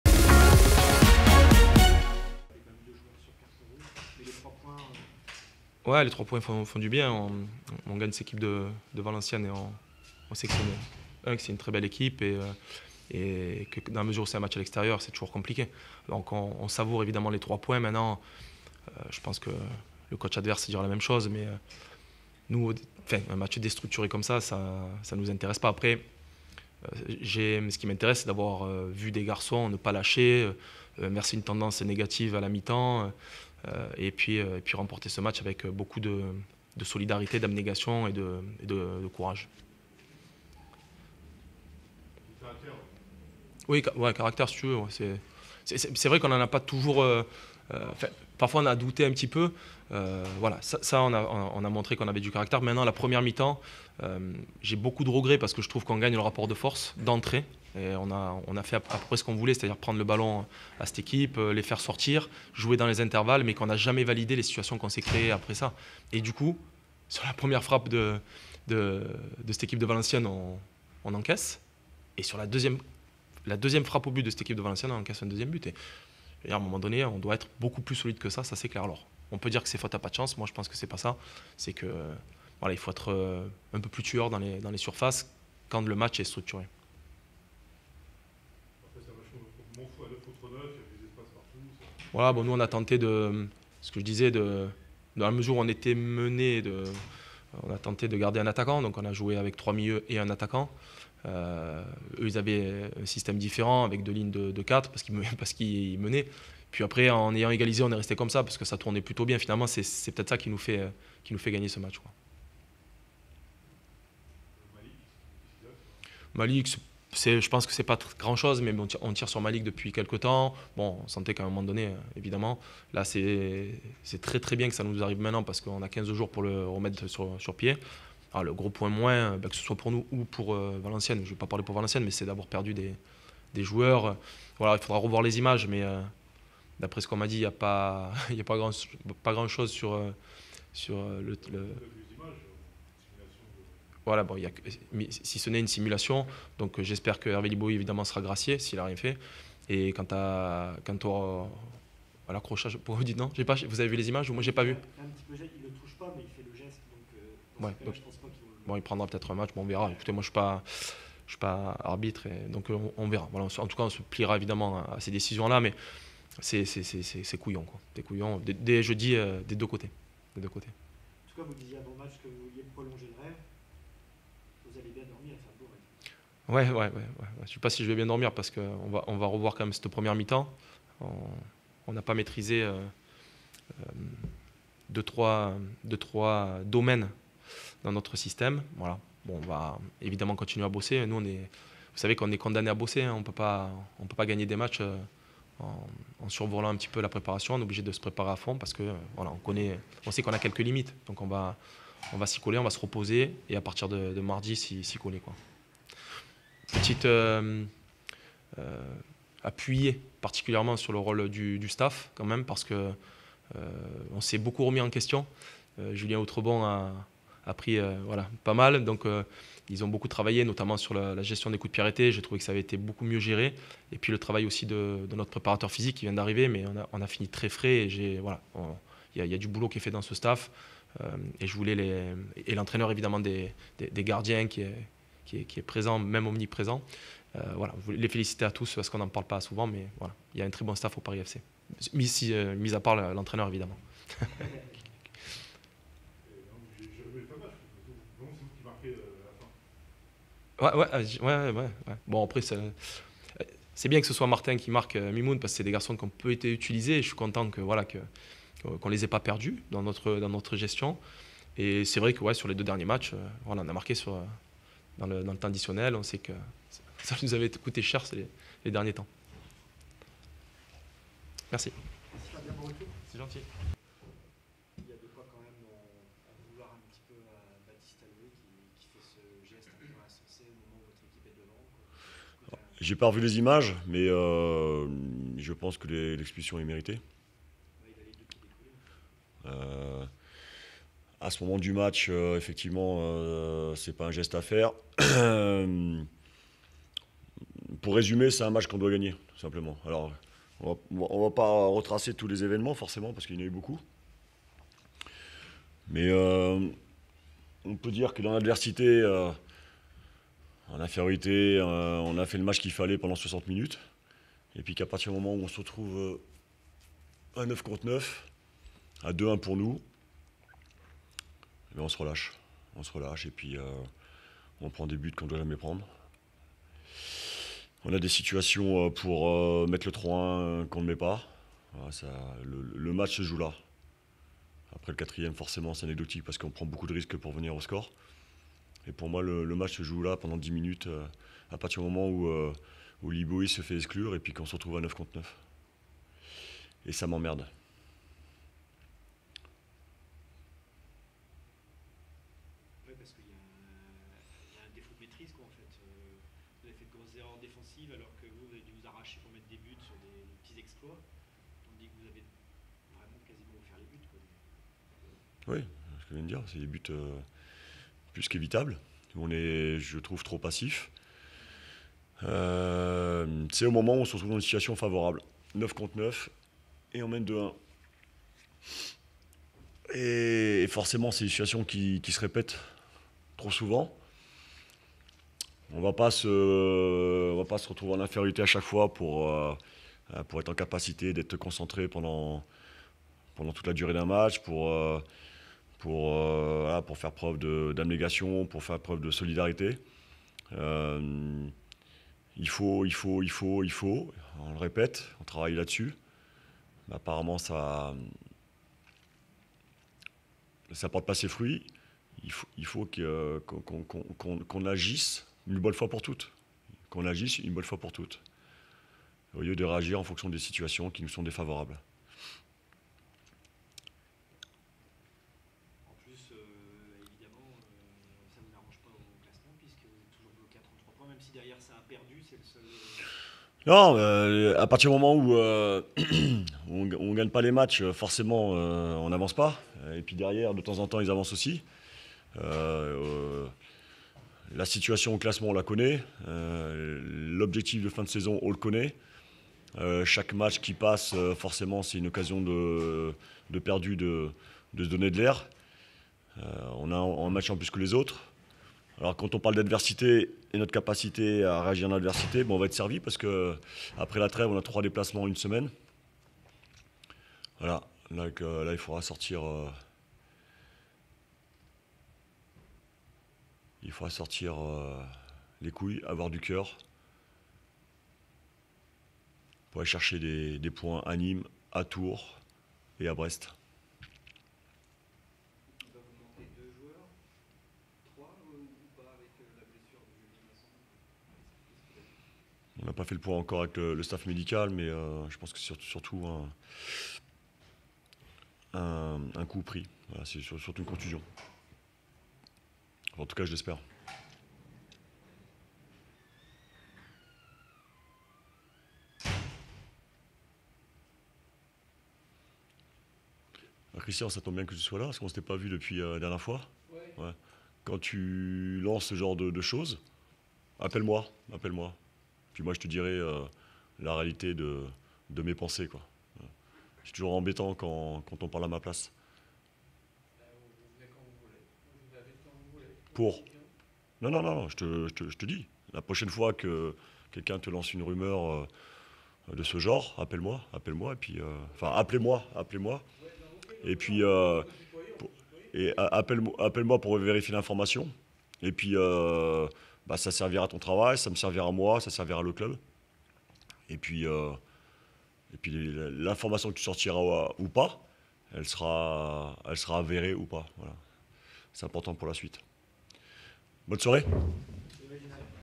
Conférence de presse Valenciennes FC - Paris FC ( 2-4 ) / 2017-18
Revivez la conférence de presse en vidéo de Valenciennes FC - Paris FC ( 2-4). Les réactions  d'après-match-match de Valenciennes FC et Paris FC.\nDomino&#039;s Ligue 2 - Saison 2017/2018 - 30ème journéeStade du Hainaut - vendredi 16 mars 2018

Buteurs : Saliou CISS (24&#039; - Valenciennes FC) / Malik TCHOKOUNTE (40&#039; - Paris FC) / Gaëtan ROBAIL (45&#039; +2 - Valenciennes FC) / Samuel YOHOU (60&#039; - Paris FC) / Dylan SAINT-LOUIS (66&#039; - Paris FC) / Martin MIMOUN (90&#039; +5 - Paris FC)
Cartons rouges/exclusions : Baptiste ALOE (36&#039; ) / Tony MAURICIO (37&#039; ) / Redouane KERROUCHE (37&#039; ) / Hervé LYBOHY (53&#039; )